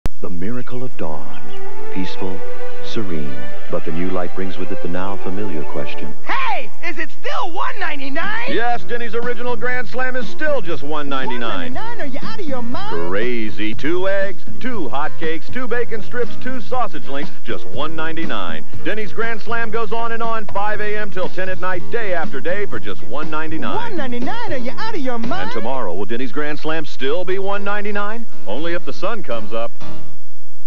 VHS - Vintage Denny's Fast Food TV Commercial (1994)
This video contains a vintage Denny's TV commercial. I do not claim ownership of the content or any associated rights. This upload is intended for preservation and nostalgic purposes only.